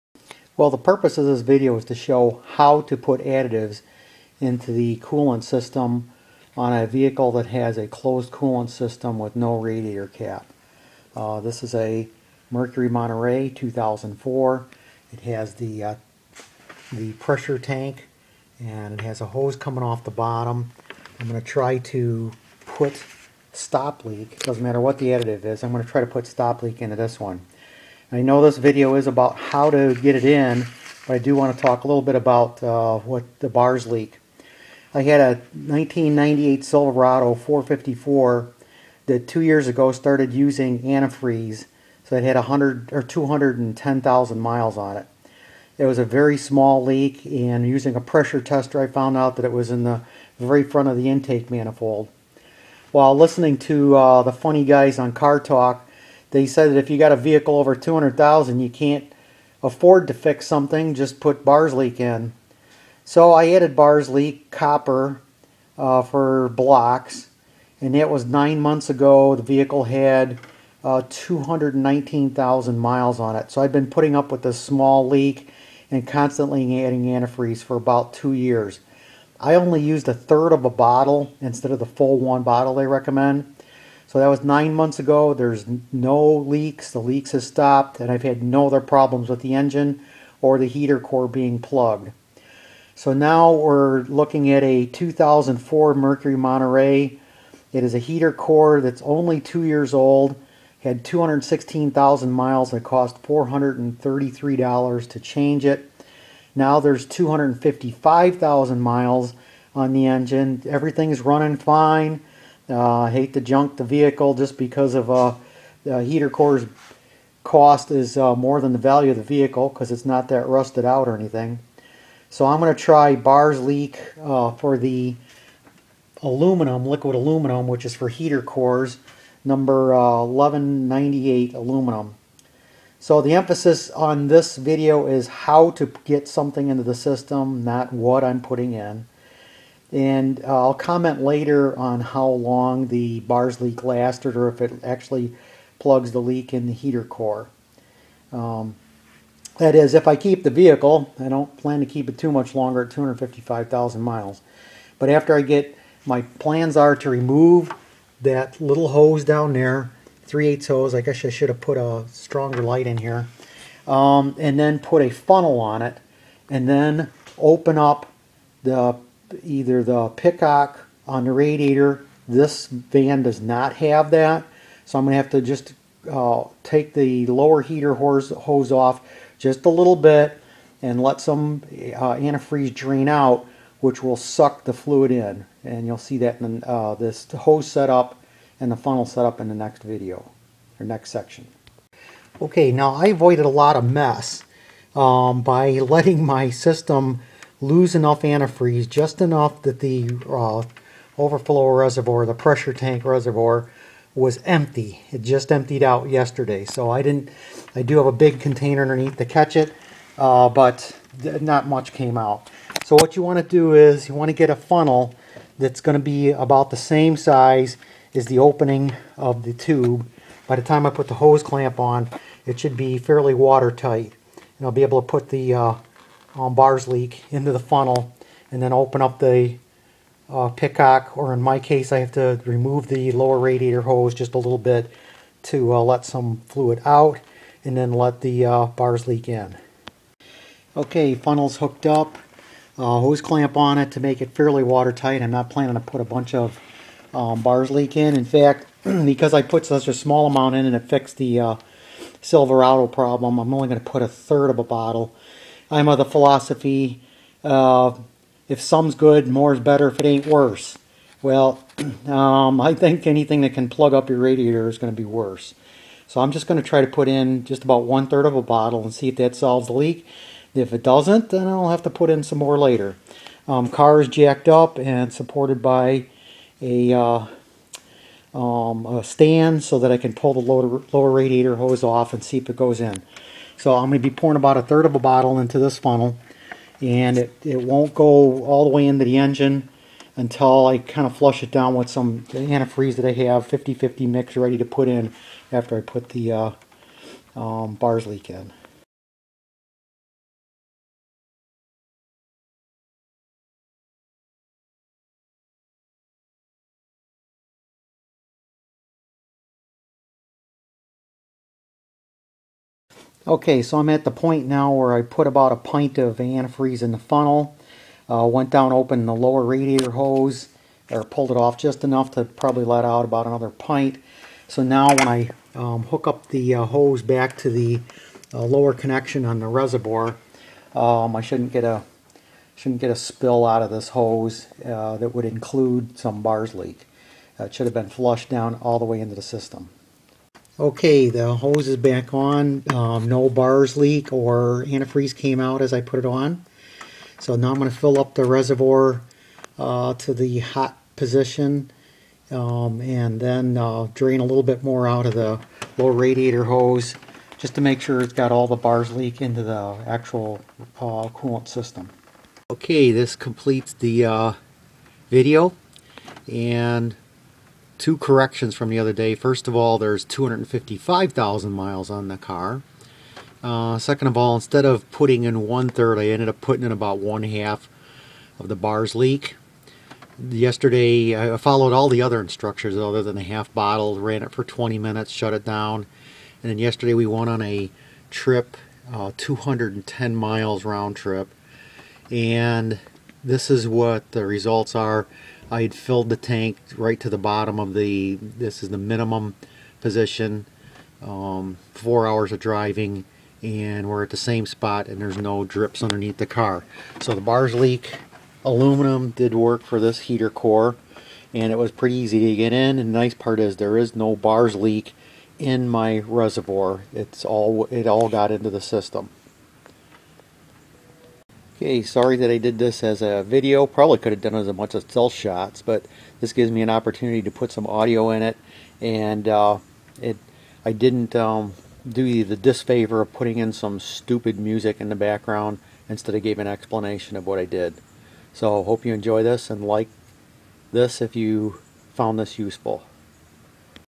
Additives to a fully sealed coolant system (no radiator cap)
How to put additives into your auto coolant system when there is no radiator cap.  This method prevents the additive from staying in the pressure tank or its connecting hose, thus failing to enter the coolant system.